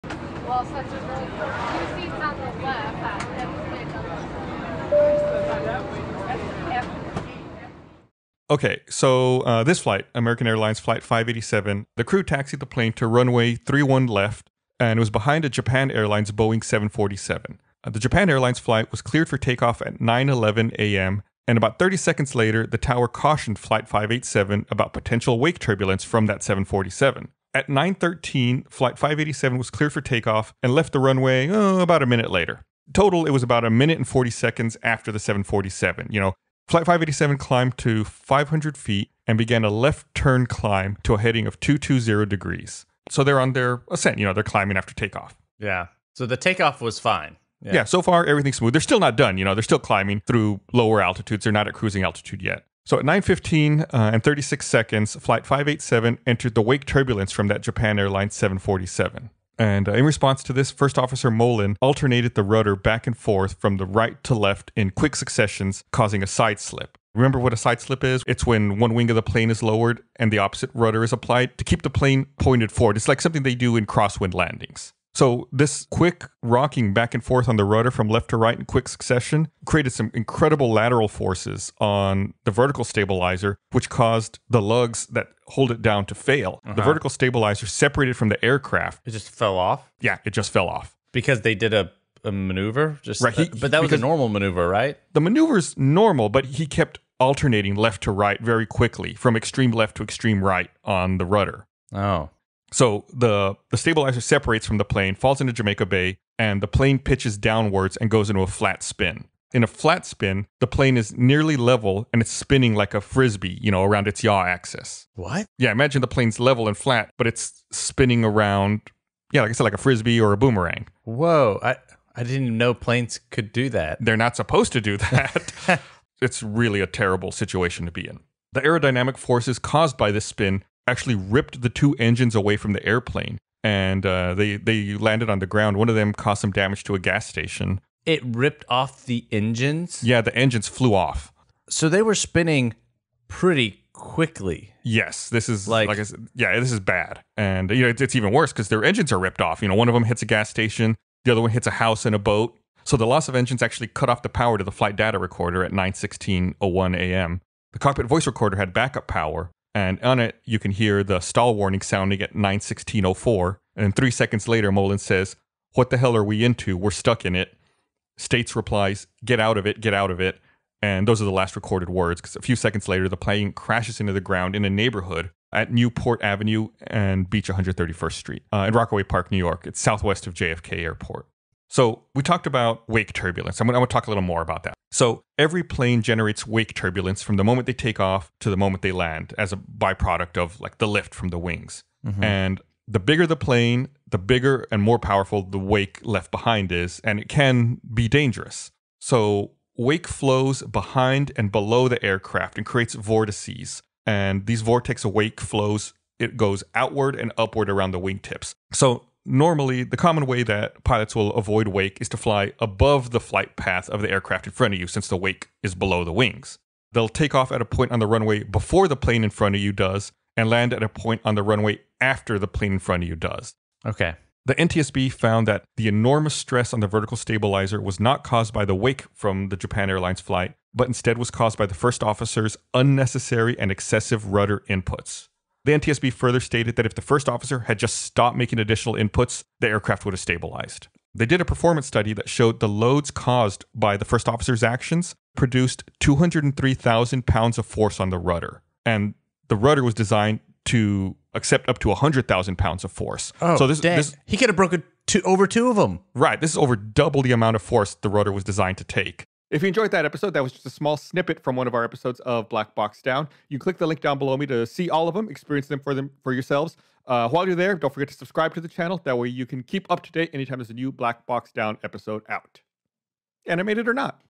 What Causes the Rear Wing to Rip Off a Plane? | Aviation Explanation
Is it wake turbulence? Pilot error? Gus and Chris discuss the cause of a plane crash into a New York neighborhood.